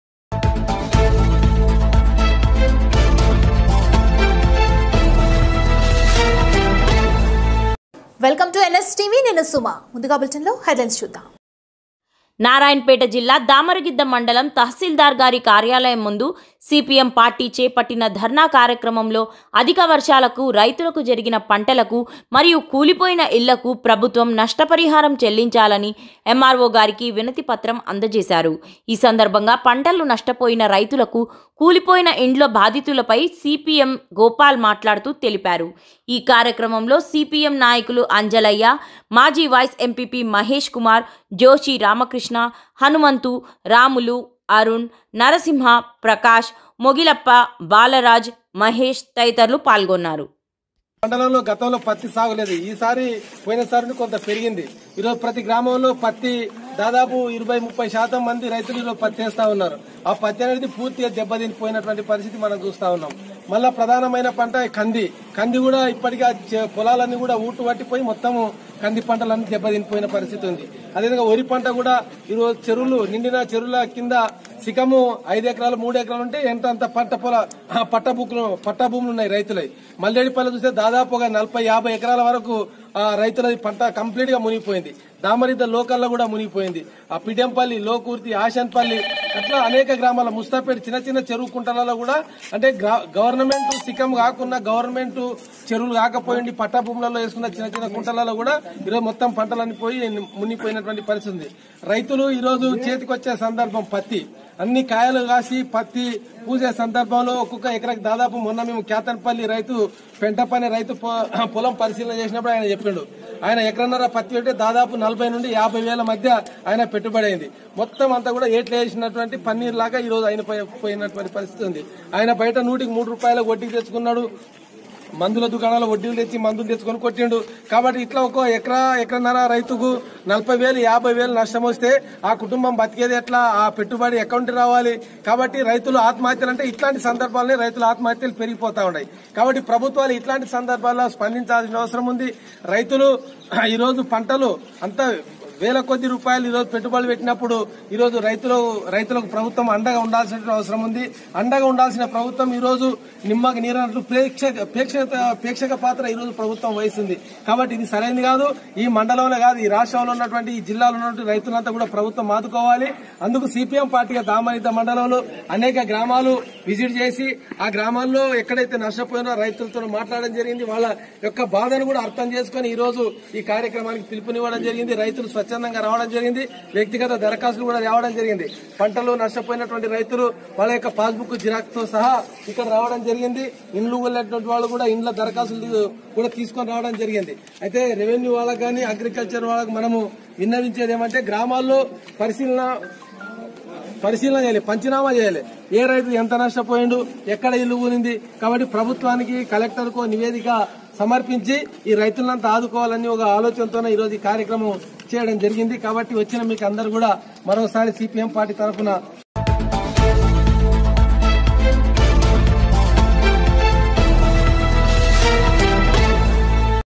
Nstv#వర్షాలకు దెబ్బతిన్న పంటలకు,కూలి పోయిన ఇళ్లకు ప్రభుత్వం నష్టపరిహారం చెల్లించాలి - CPM
Nstv#నారాయణపేట జిల్లా,దామరగిద్ద||వర్షాలకు దెబ్బతిన్న పంటలకు,కూలి పోయిన ఇళ్లకు ప్రభుత్వం నష్టపరిహారం చెల్లించాలి - CPM